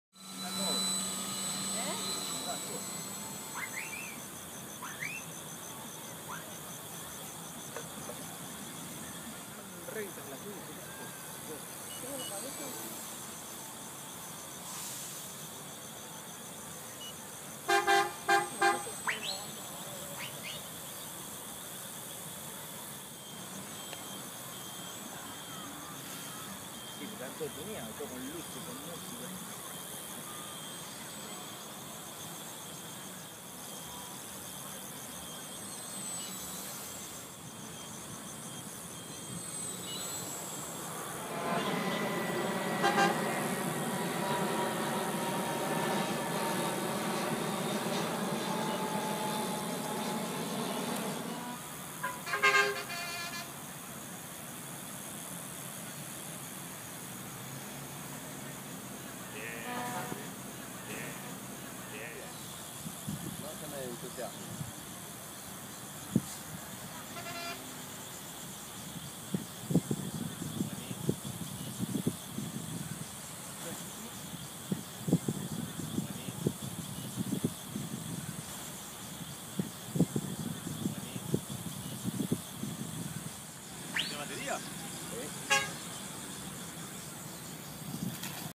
Vuelo nocturno Vicente Lopez
Segundo vuelo nocturno con el Ash One en Vicente Lopez. Luces armadas por Gabriel.